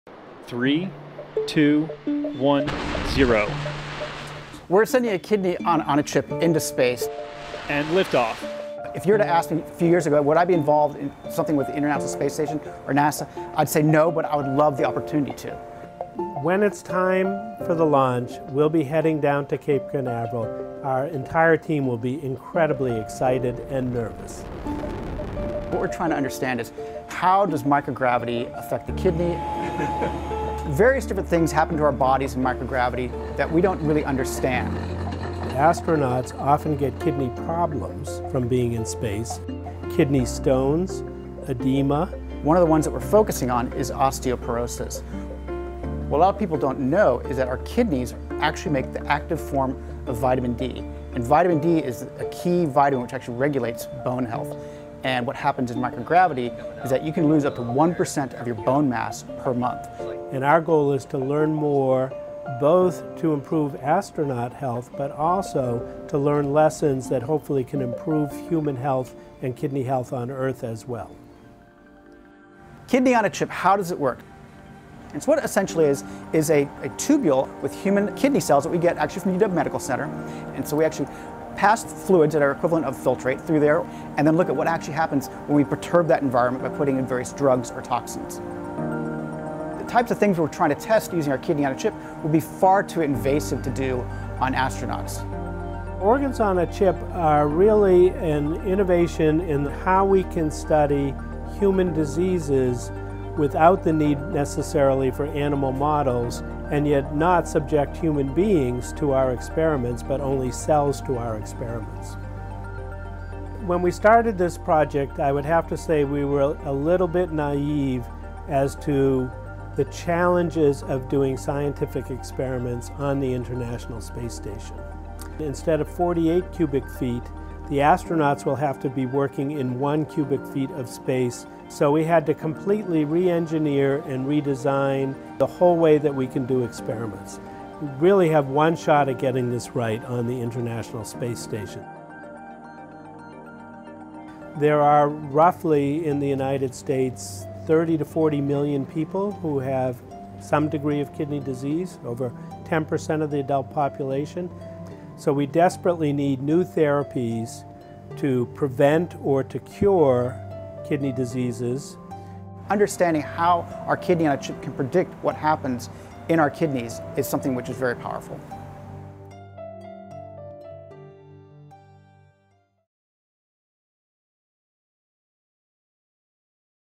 UW scientists study effect of micro-gravity on kidneys
UW scientists are prepping A 'kidney-on-a-chip' experiment at Cape Canaveral, Florida, awaiting a shuttle launch that will take the 'chips' into space. At an altitude of 250 miles, astronauts will help study how reduced gravity in space affects kidney physiology.